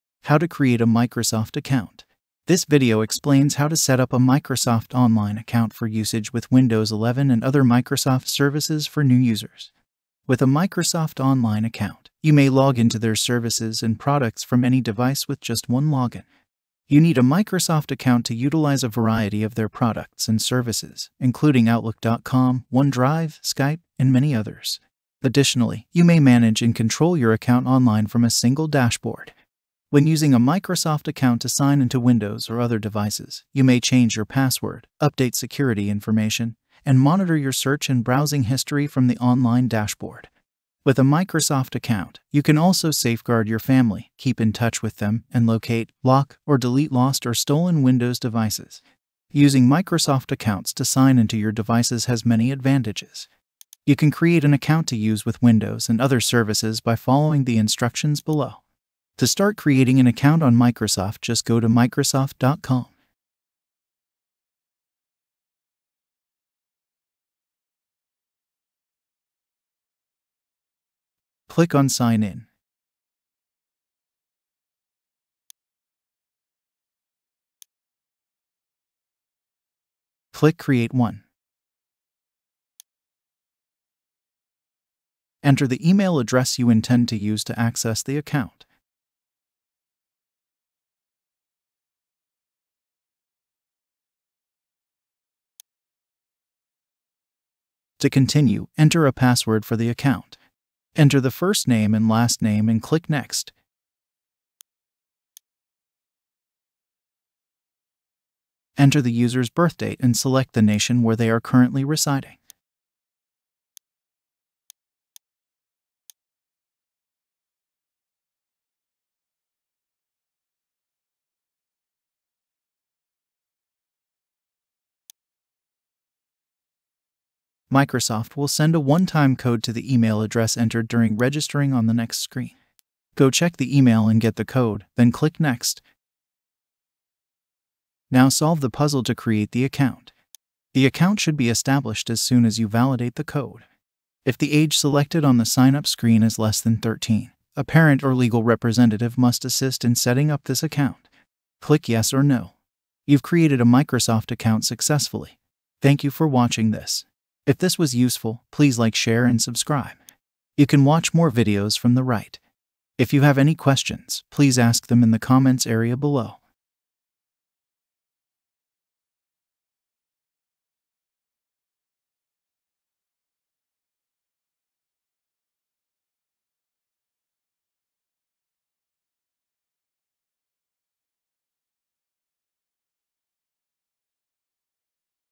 How to Create a Microsoft Account
In this video, we'll show you how to create a Microsoft account. This is the first step in using many of the features on the Microsoft website and in Microsoft products.

If you're new to the internet or new to using Microsoft products, create a Microsoft account now! This video will show you how easy it is to create a Microsoft account and start using the many great features available on the Microsoft website!

Learn in this tutorial How to Create a Microsoft Account.